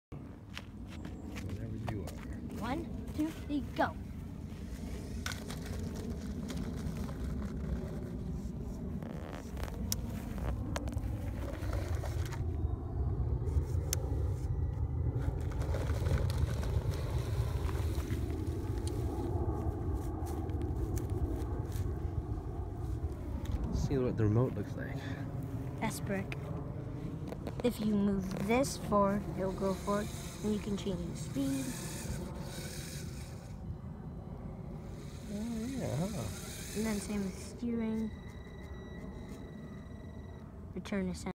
Lego Technic RC car off-road
Gavin test-driving his Lego Technic RC car/build #481 ha!